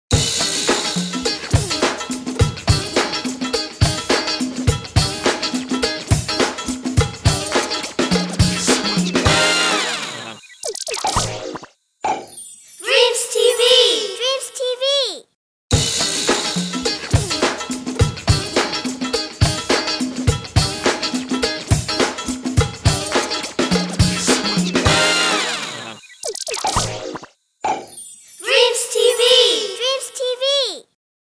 Hình hiệu kênh HTV3 2018 DreamsTV
HTV3 được thành lập bởi Ban Biên tập các kênh truyền hình số và cáp - Đài Truyền hình Thành phố Hồ Chí Minh (HTV) và bắt đầu phát sóng thử nghiệm từ ngày 1 tháng 10 năm 2003.